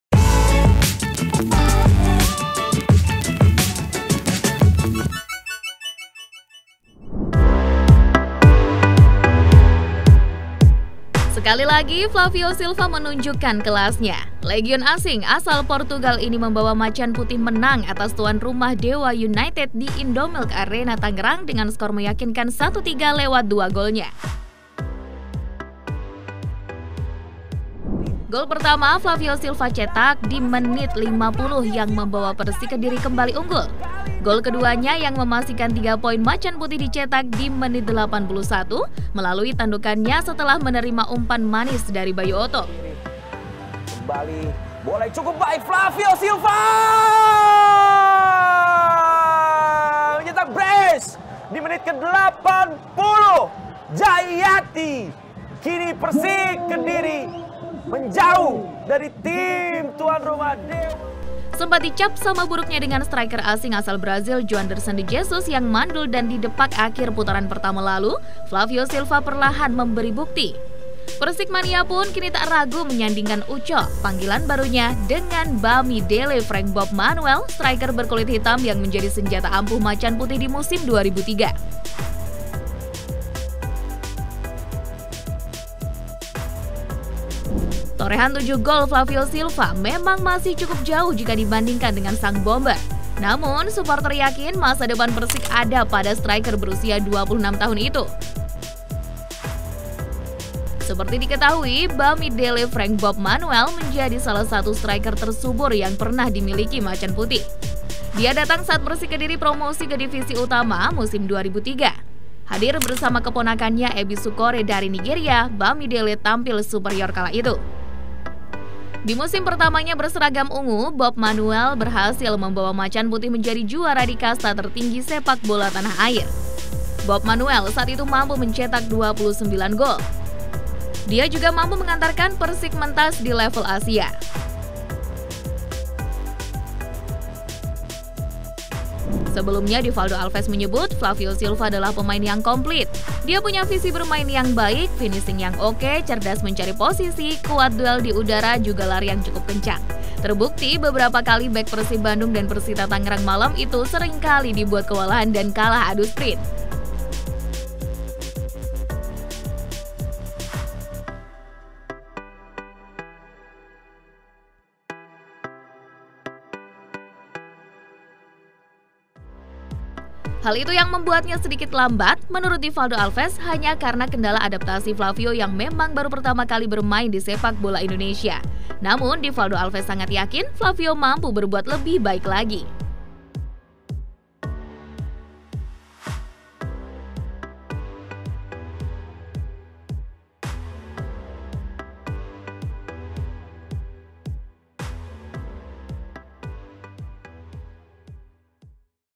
Persik Mania Sandingkan Flavio Silva dengan Bob Manuel, Bomber 2003
Sekali lagi, Flavio Silva menunjukkan kelasnya. Legiun asing asal Portugal ini membawa Macan Putih menang atas tuan rumah Dewa United di Indomilk Arena Tangerang dengan skor meyakinkan 1-3 lewat dua golnya.

Gol pertama Flavio Silva dicetak di menit 50 yang membawa Persik Kediri kembali unggul. Gol keduanya, yang memastikan tiga poin Macan Putih dicetak di menit 81 melalui tandukannya setelah menerima umpan manis dari Bayu Otto.

Sempat dicap sama buruknya dengan striker asing asal Brazil Joanderson de Jesus yang mandul dan didepak akhir putaran pertama lalu, Flavio Silva perlahan memberi bukti.